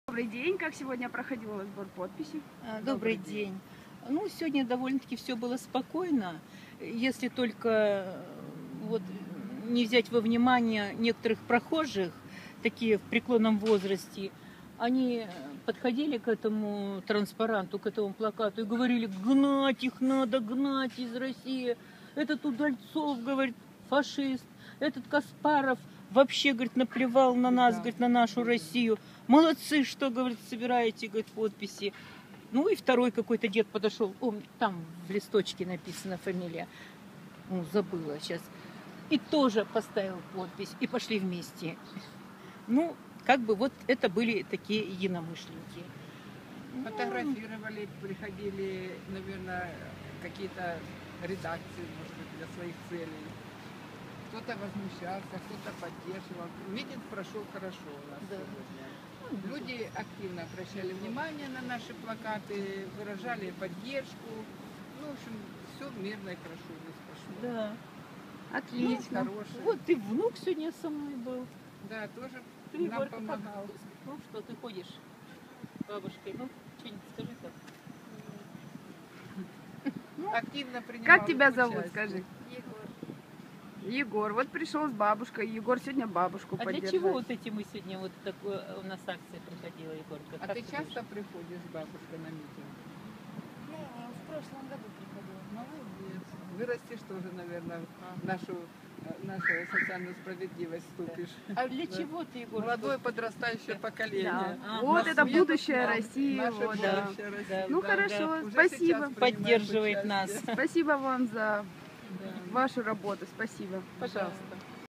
Люди говорят - Удальцов фашист! 16.07.13г
На улицах Краснодара активистами движения "Социальная справедливость" продолжается сбор подписей в поддержку законодательной инициативы о лишении гражданства лиц, ведущих антигосударственную деятельность.